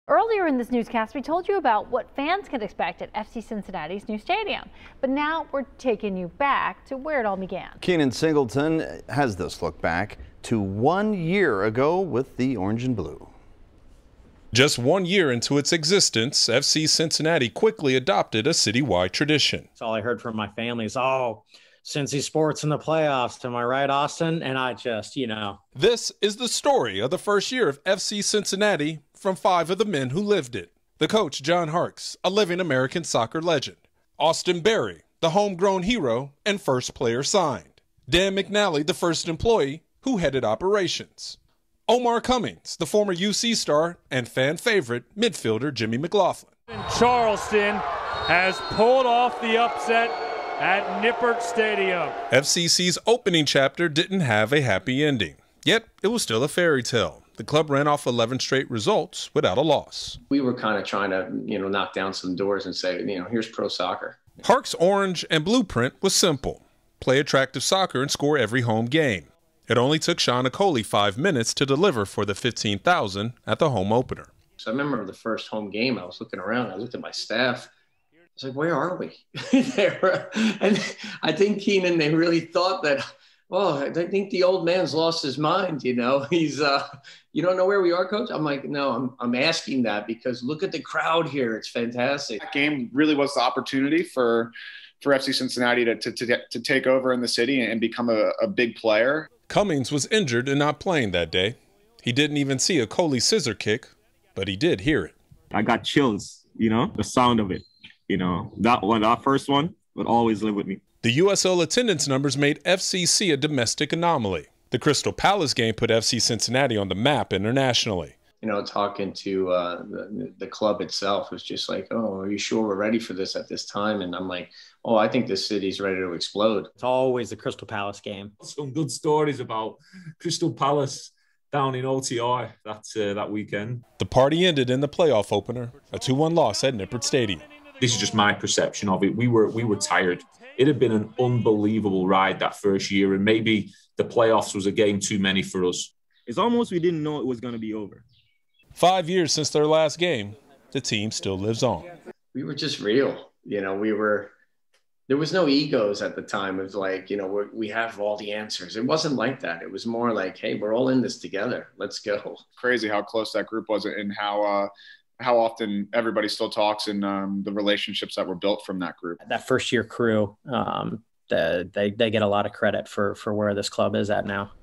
The First Year of FC Cincinnati Part 2
Just one year into its existence, FC Cincinnati quickly adopted a city-wide tradition. This is some of that story of the first year of FC Cincinnati from five men who lived it.